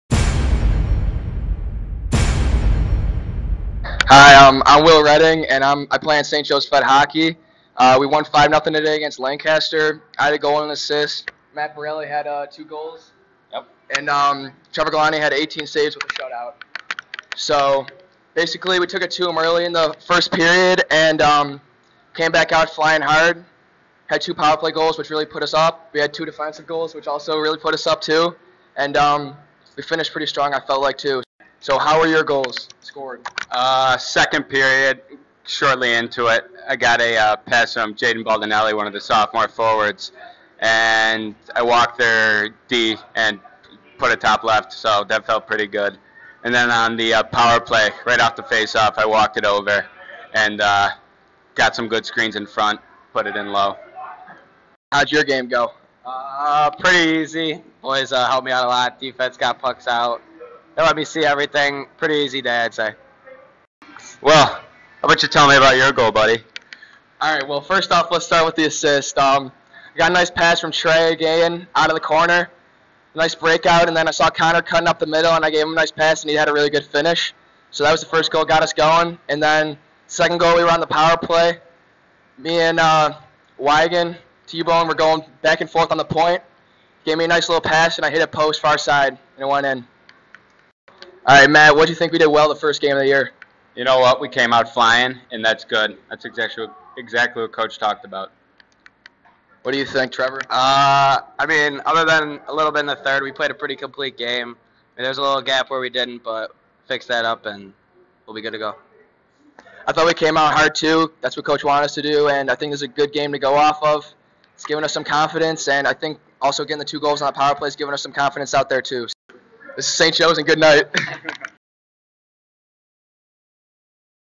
St. Joe’s Post-Game 11/25/18
Trevor Galante, Will Redding, Matt Barrali. 5-0 win against Lancaster.